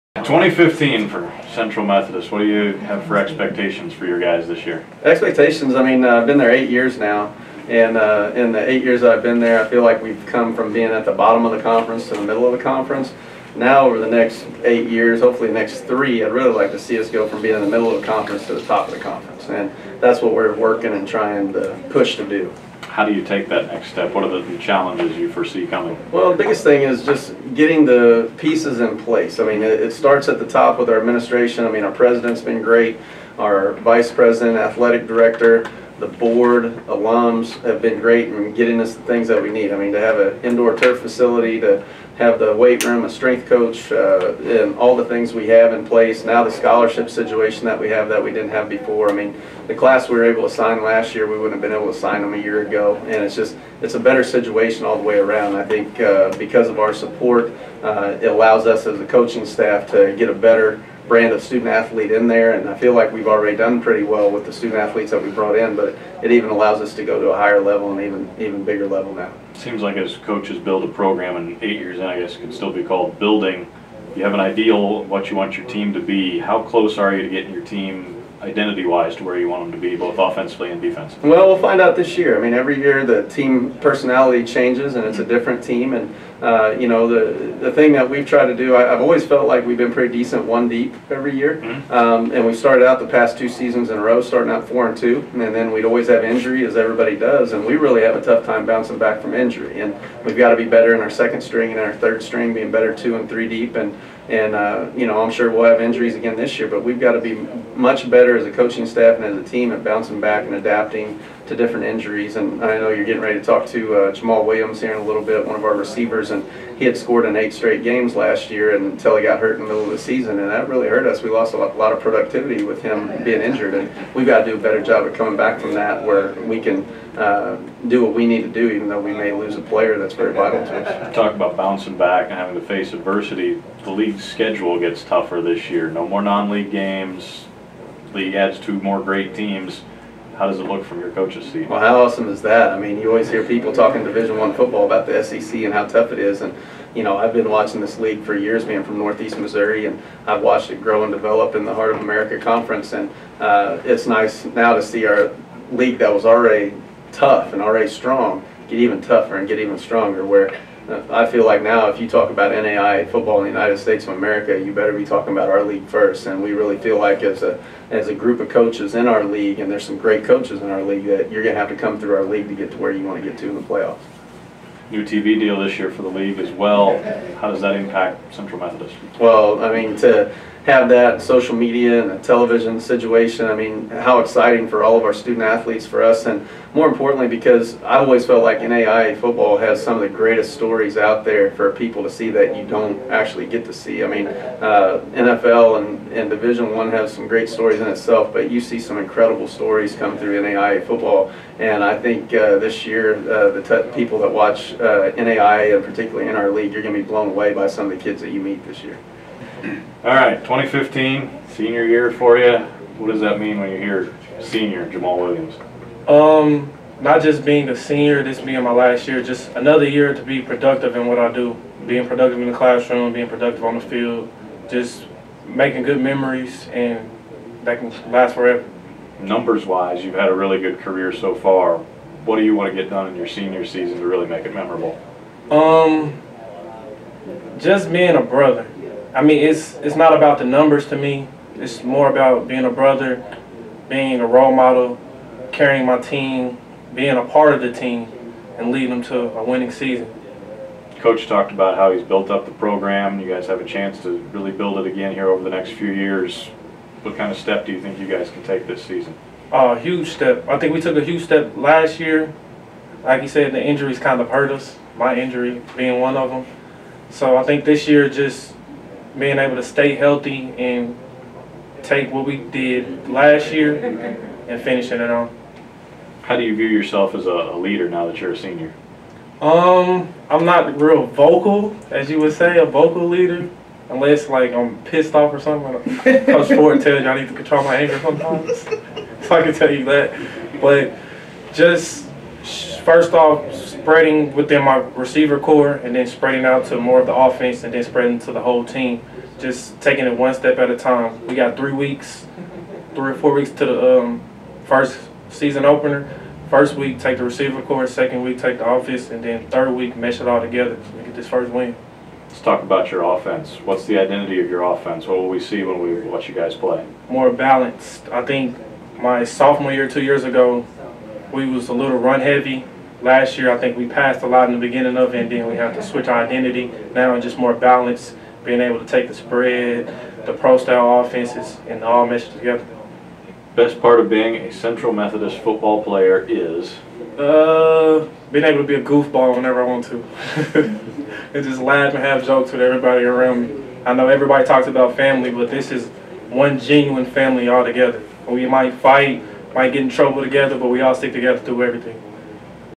HeartFB15 Media Day - Central Methodist
Interviews with head coach Jody Ford, senior WR Jamall Williams